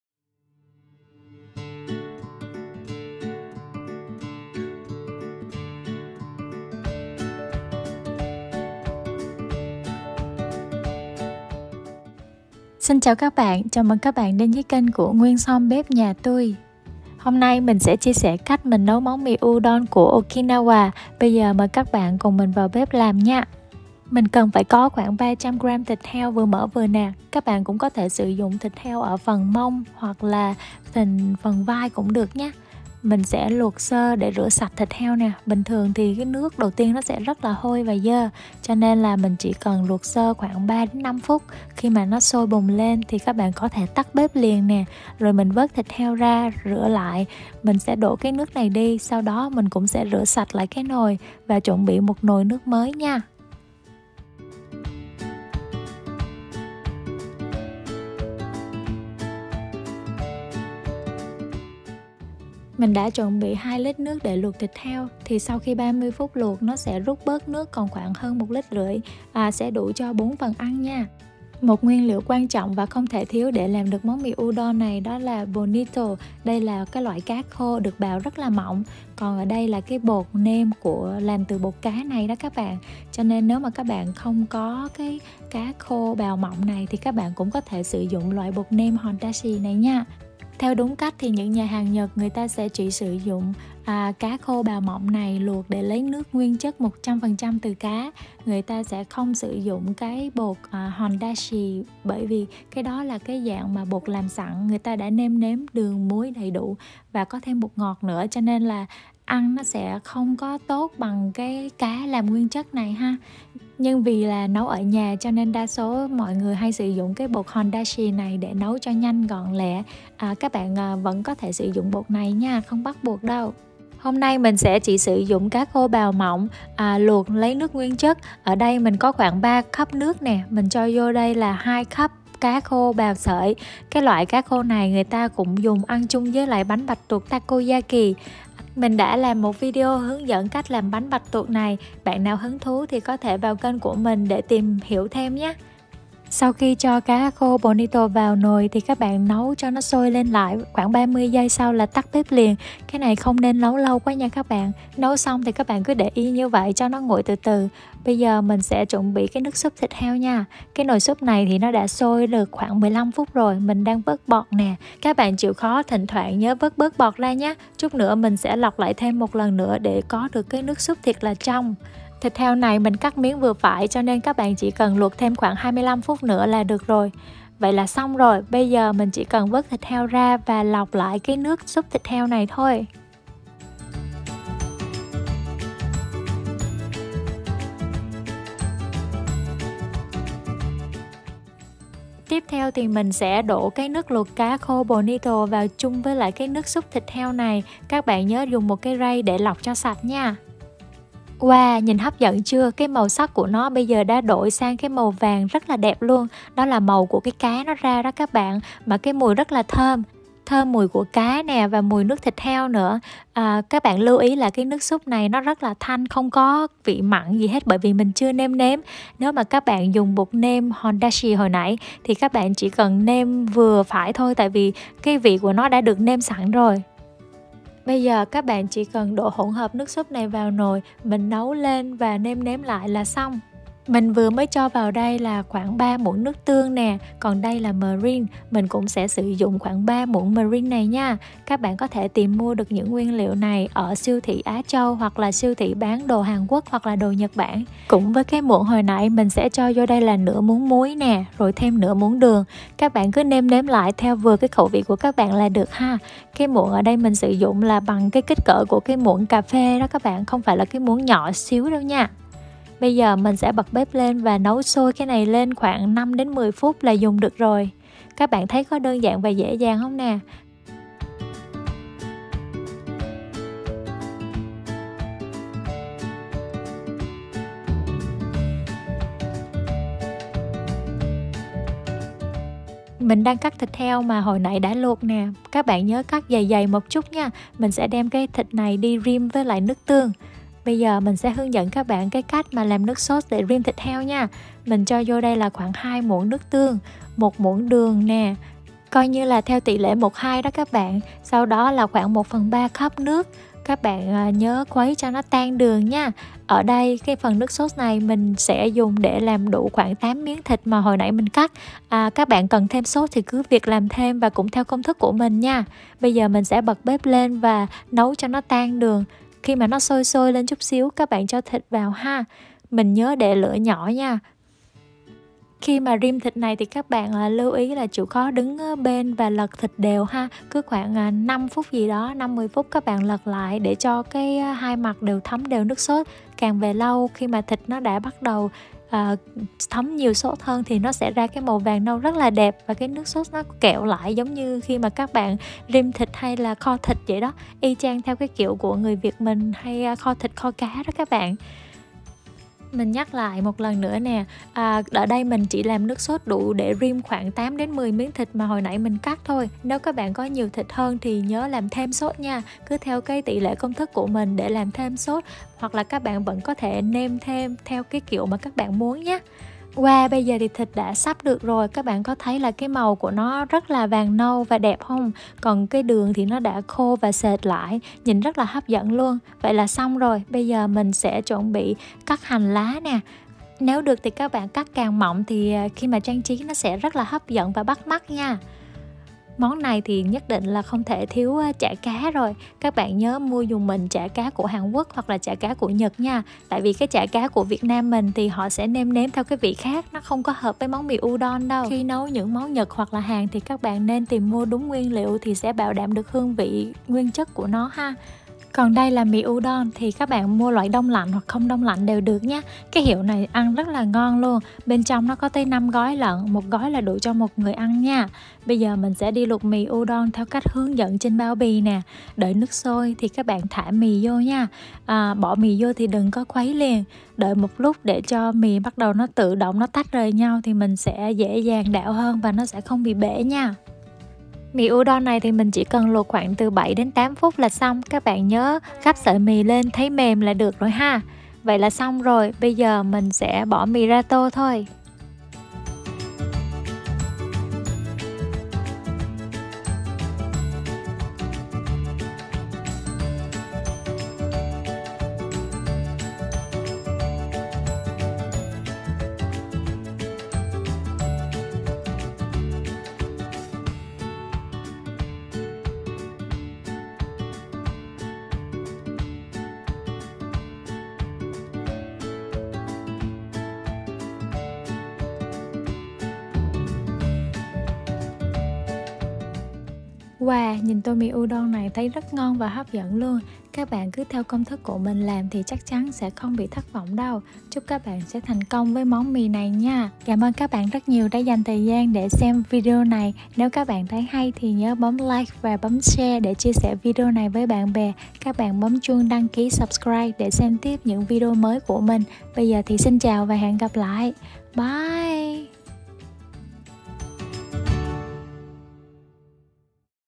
Cách nấu mì Udon Nhật Bản rất đơn giản và dễ làm tại nhà - bánh canh kiểu Nhật- mì udon Okinawa
Ẩm thực mì udon của Nhật rất phong Phú, có rất nhiều loại, hôm nay các bạn thử làm món mì udon okinawa này để thưởng thức xem vị nó thế nào nè, ăn một lần là mê hoài hoài luôn!!